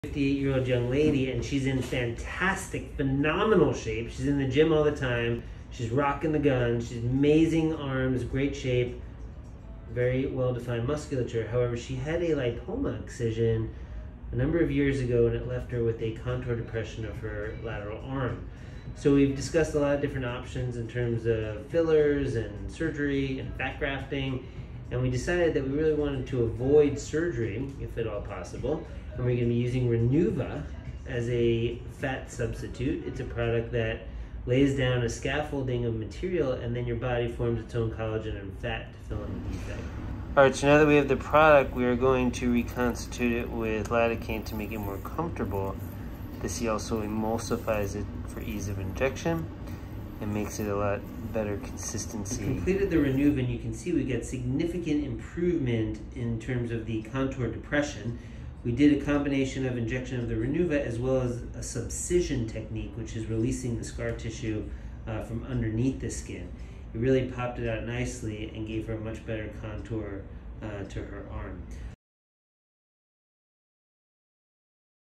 Learn How Dr. Farber Uses Renuva As A Fat Substitute
Is volume loss becoming an issue for you? Renuva can restore your volume loss as it is an injectable treatment that replaces age-related fat loss with your body’s natural fat.